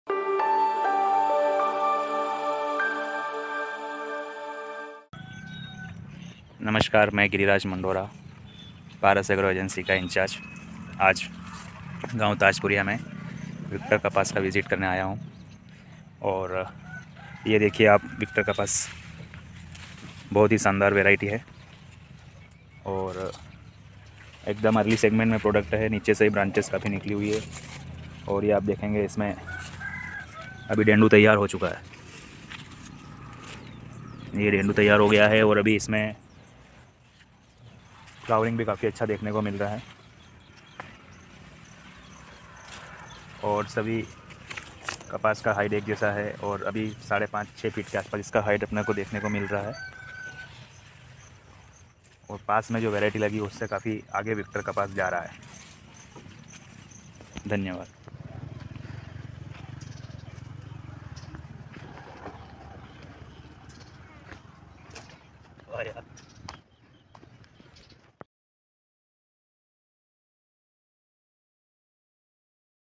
Victor BGII DCH Hybrid Cotton Seeds | Paras Beej
India's selling hybrid cotton seed variety "Victor BG II" in the long-staple cotton segment with bollgard 2 technology gives both a high yield and better results in irrigated and rain-fed conditions.

Location: Madhya Pradesh